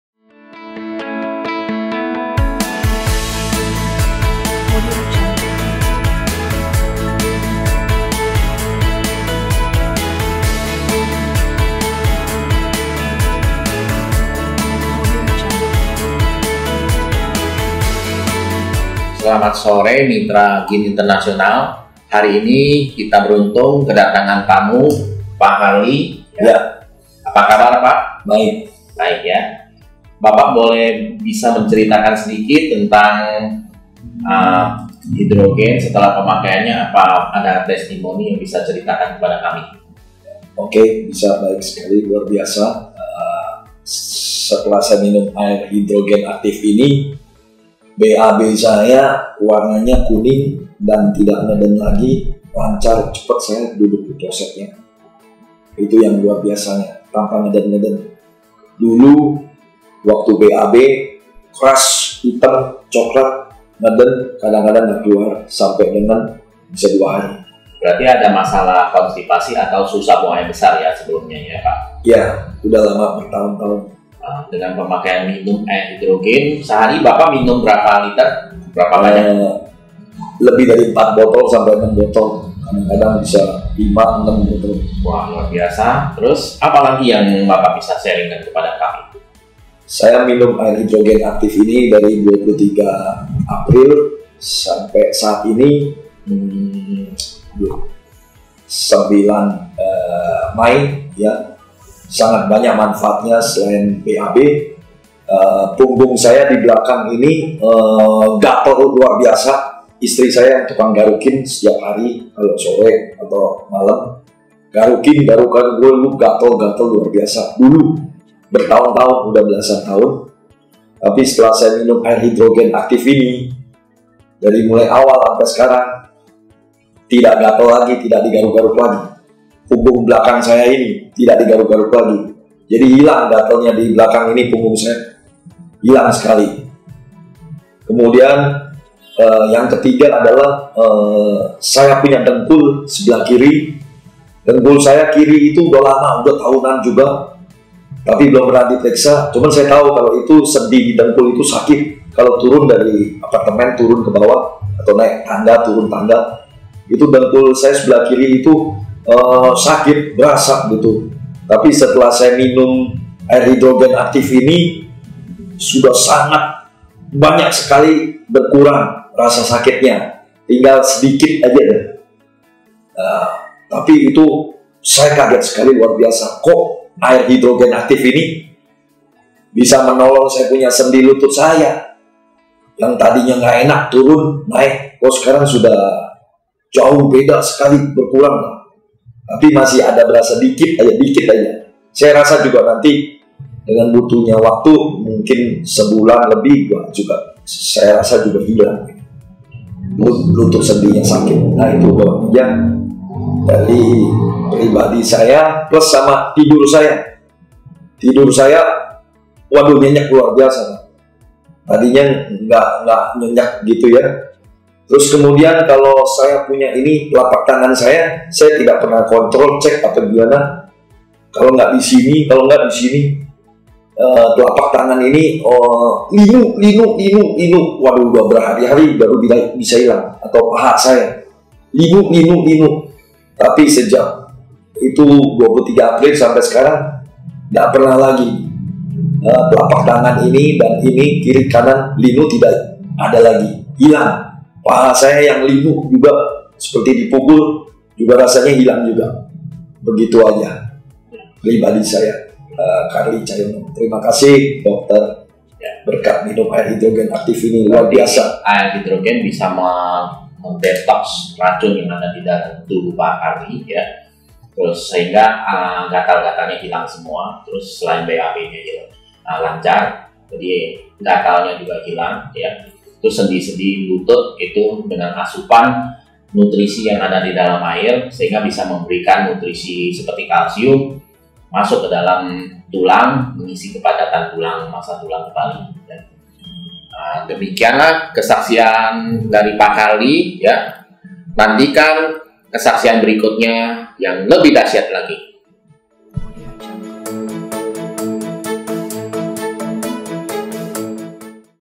Hydrogin - Gatal serta nyeri pada dengkul hilang hanya dalam waktu 2 minggu saja!
Setelah mengkonsumsi ari Hydrogin Dynamic, secara rutin 5-6 botol selama 2 minggu, konstipasinya membiaki dan gatal serta nyeri pada dengkul pun sudah hilang, selain itu juga kualitas tidur Pak Karli juga jauh lebih baik.
Cara Praktis membersihkan botol Hydrogin Dynamic! Check this out!